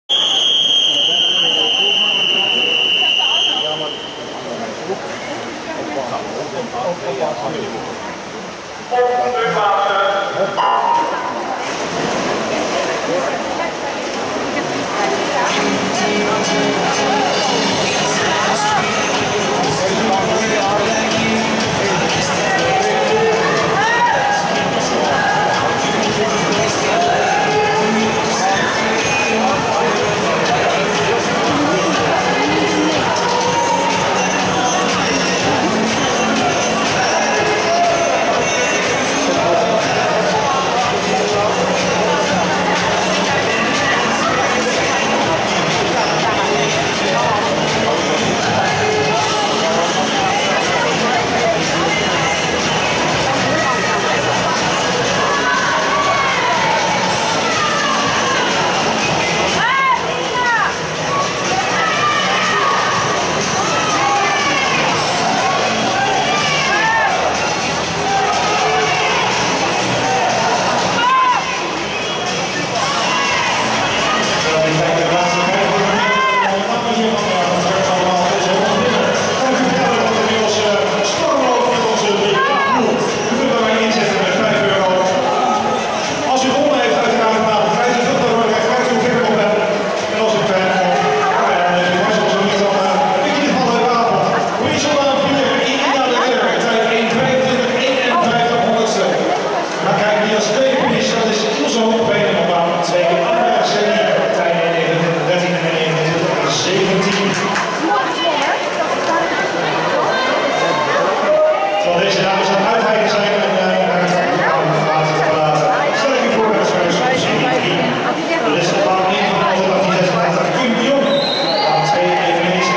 Irina 100 vlinder jaargangfinale Dordrecht min 5 290614
Irina wint haar serie en wordt 4e op de 100 vlinder in 1.25.51.